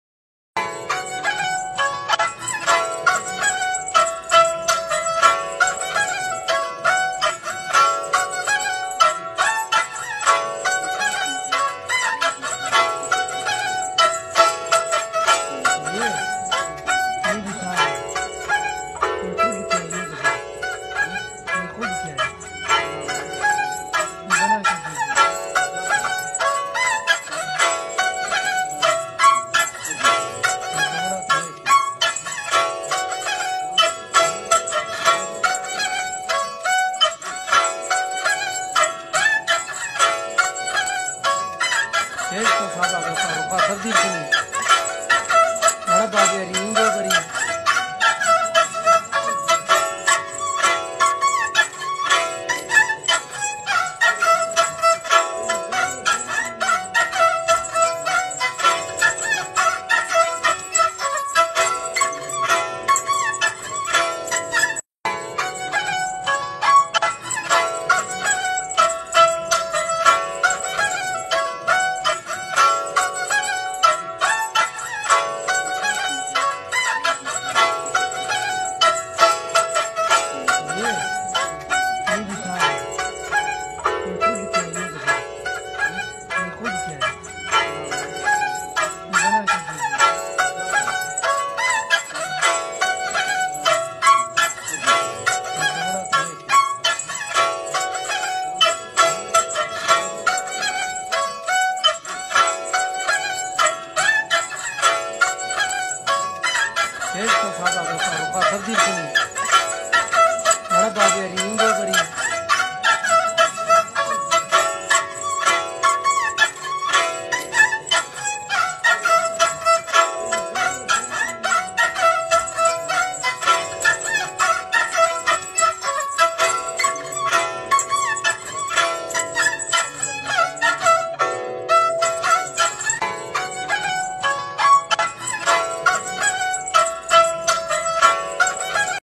Balochi Traditional method of smoking with stone & Steel combination
It's an old method of burning fire ,in those time their was no Matchs,